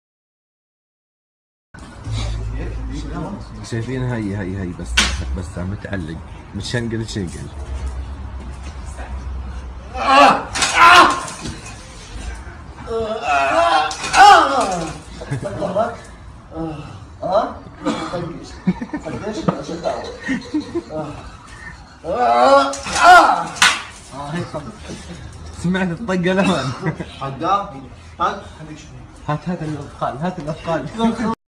Вытяжение (тракция) позвоночника | ШОК | Spinal Traction
18+ Слабонервным не смотреть! Страшное видео! Оказывается и такое бывает! Разве это лечение?